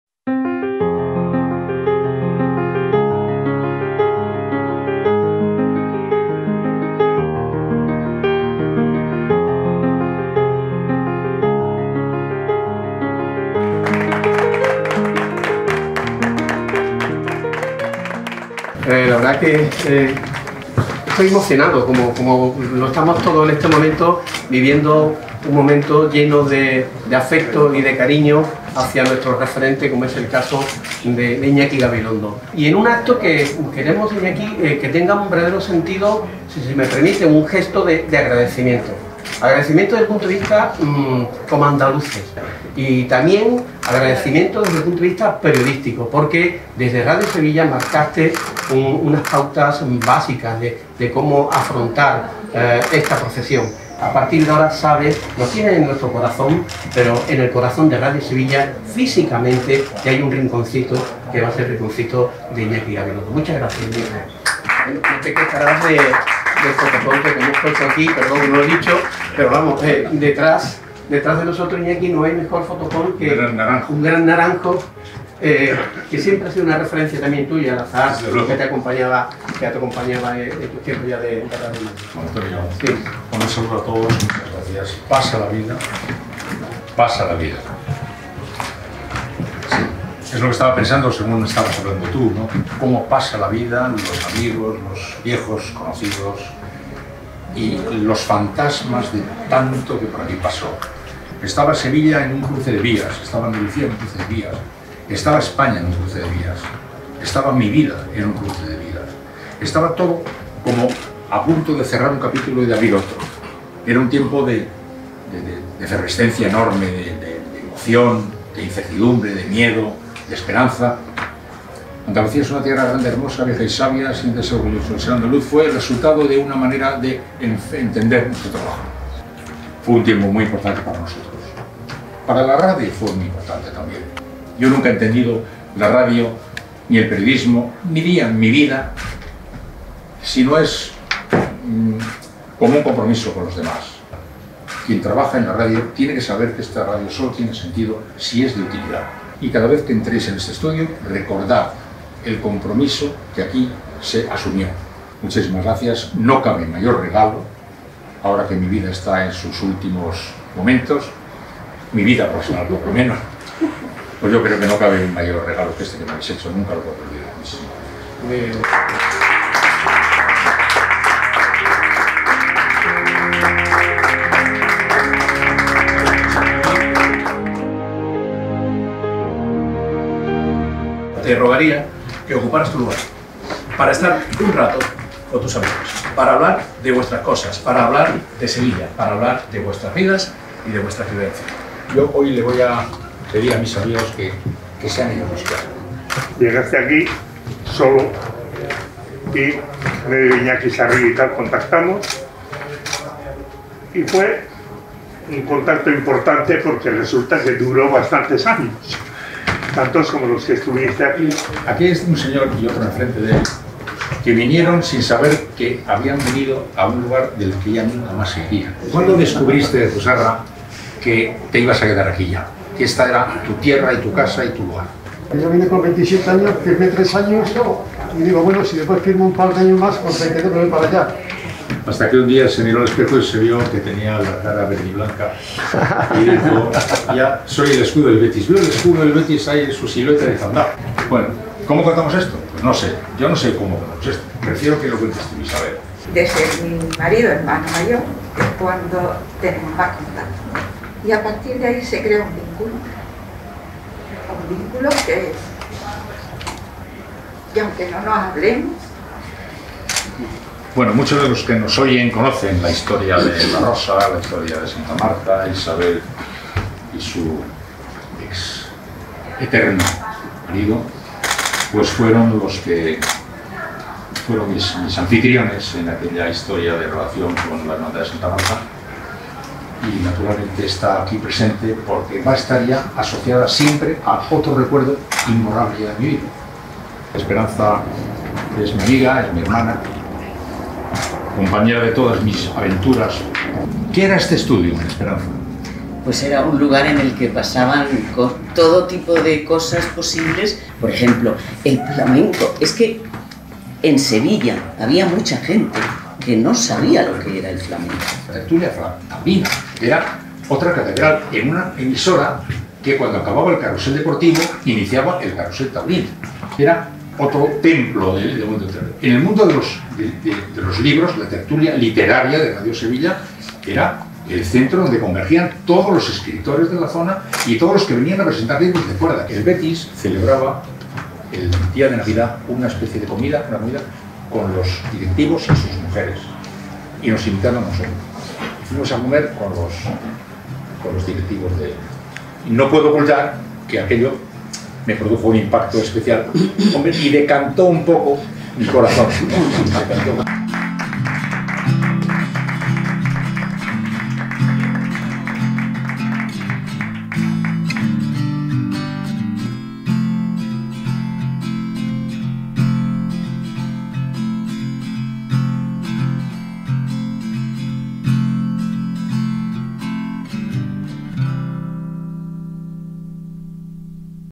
Inauguración estudio Iñaki Gabilondo de Radio Sevilla
El maestro de periodistas Iñaki Gabilondo ha presidido este miércoles la inauguración de los trabajos de remodelación del estudio principal de Radio Sevilla,que desde hoy lleva su nombre.
Durante el acto, Iñaki, acompañado por Antonio Yélamo Crespillo, director de Radio Sevilla y de la Cadena SER en Andalucía, ha descubierto un azulejo de cerámica trianera.
Video de  Radio Sevilla.
Sevilla,9 de junio de 2021